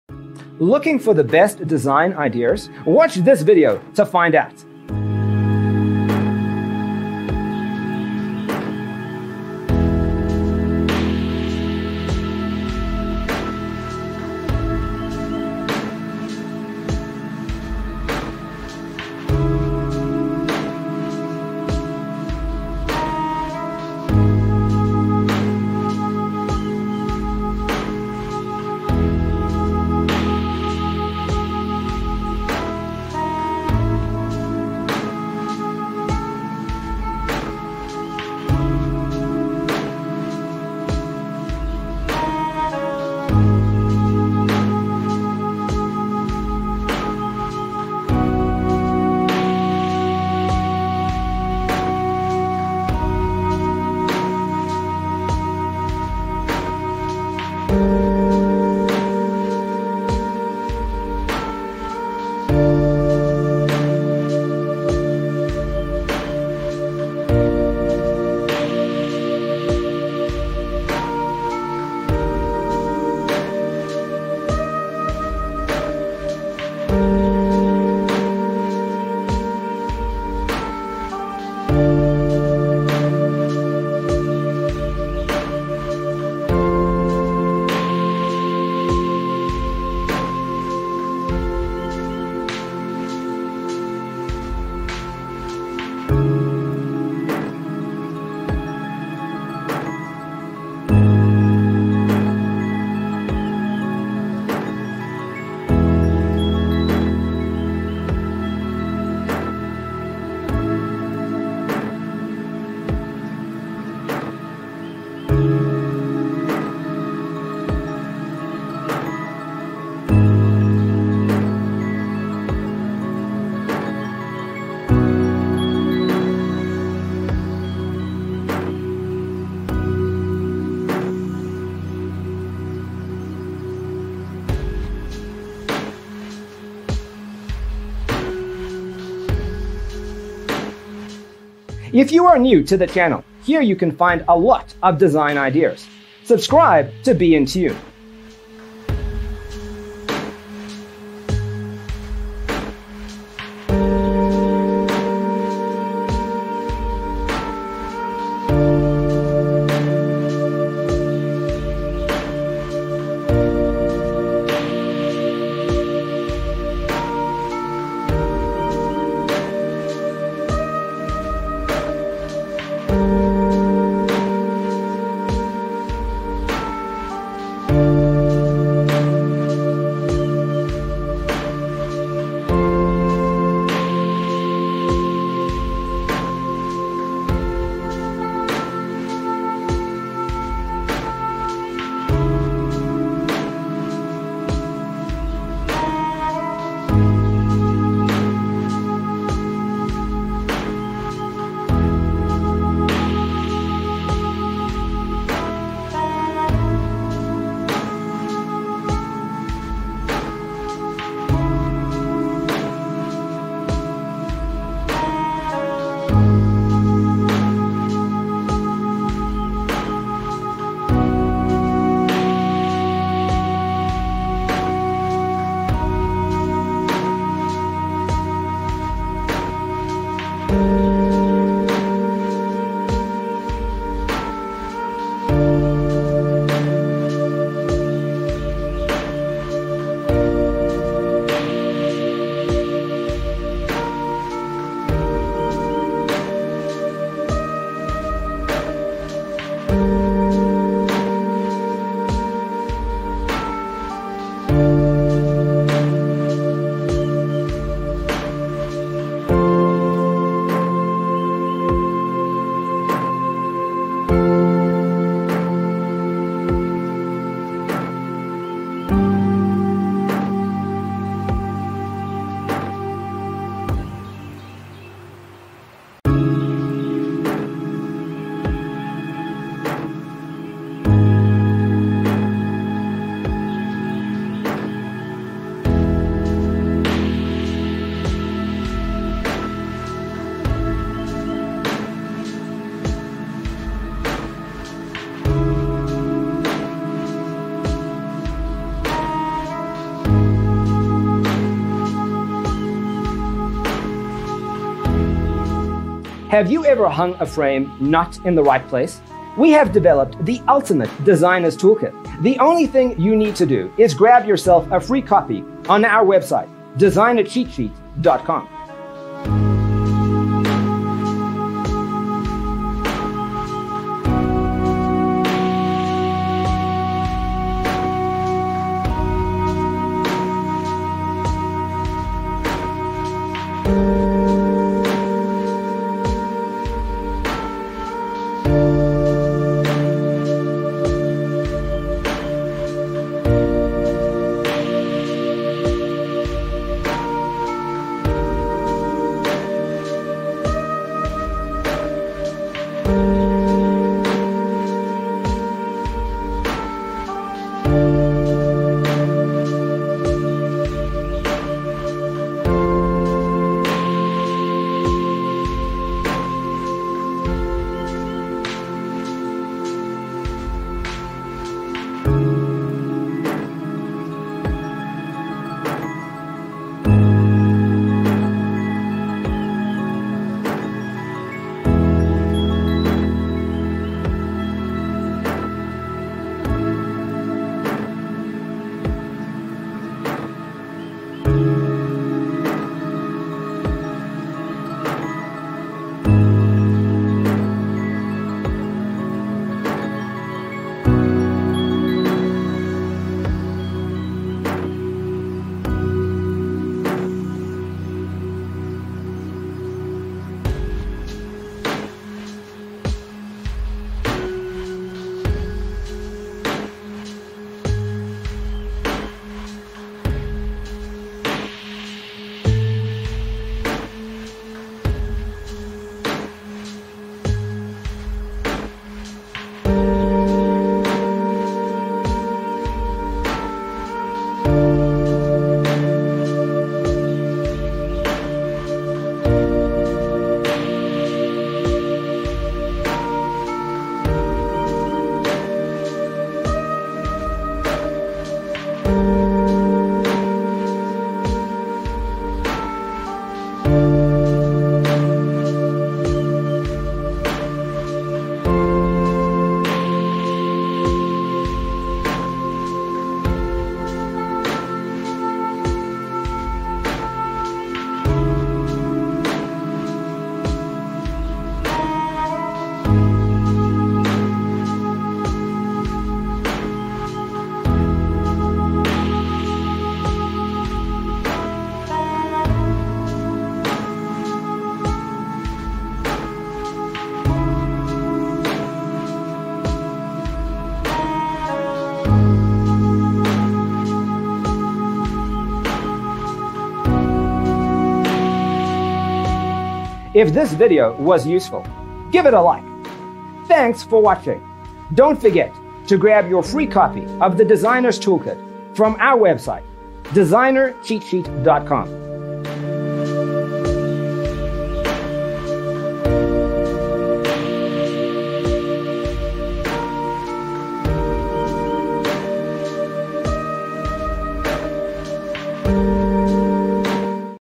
75 Limestone Floor And Ceramic Tile Bedroom Design Ideas You'll Love 🔴
0:00:00 - Intro
0:00:12 - Limestone Floor And Ceramic Tile Bedroom Design Ideas You'll Love: Inspirations 1 to 25
0:03:03 - Limestone Floor And Ceramic Tile Bedroom Design Ideas You'll Love: Inspirations 26 to 50
0:06:07 - Limestone Floor And Ceramic Tile Bedroom Design Ideas You'll Love: Inspirations 51 to 75